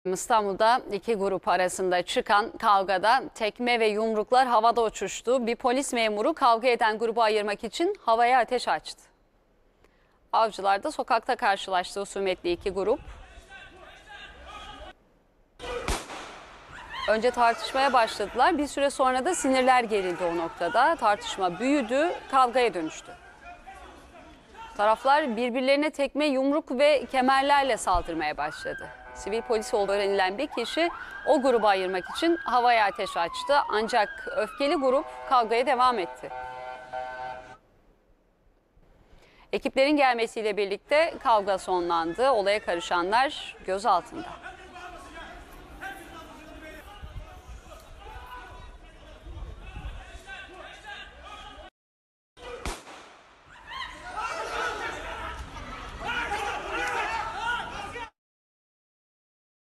Avcılar’da iki grup arasında kemerli, tekmeli ve yumruklu kavga | A Haber
Olay, geçtiğimiz gün saat 17.00 sıralarında Avcılar Şükrübey'de yaşandı. İddiaya göre, sokakta karşılaşan iki grup arasında henüz bilinmeyen bir nedenle tartışma çıktı. Tartışmanın büyümesiyle sinirlerine hakim olamayan taraflar birbirlerine tekme, yumruk ve kemerle saldırdı. Kavgayı gören vatandaşlar, tarafları ayırmak istese de başarılı olamadı.

SİVİL POLİS HAVAYA ATEŞ AÇTI, ÖFKELİ GRUP YİNE DE AYRILMADI

Kavganın büyümesiyle sokakta bulunan vatandaşlar, iki grubu ayırmak için uzun süre uğraştı. Karşılıklı birbirini darp eden grup, sakinleşmeyince sivil polis olduğu öğrenilen bir kişi, tabancayla havaya ateş açtı. Sivil polisin olaya müdahalesine bile aldırış etmeyen öfkeli grup, kavgaya devam etti. Ekip aracının sokağa gelmesinin ardından grup üyeleri sokak aralarına kaçarak gözden kaybolurken, kavgaya karışanlardan bazıları ise yakalanarak gözaltına alındı.

İKİ GRUBUN BİRBİRİNE KEMERLE SALDIRDIKLARI ANLAR KAMERADA

Avcılar'da öfkeli iki grubun kavga anı güvenli kamerasına yansıdı. Görüntülerde, gruplarından birbirine ilk önce tekme ve yumruklarla saldırdığı daha sonra ise kemerleri belinden çıkartarak birbirilerini darp ettikleri ve vatandaşlarında ayırmak için uğraştıkları görülüyor.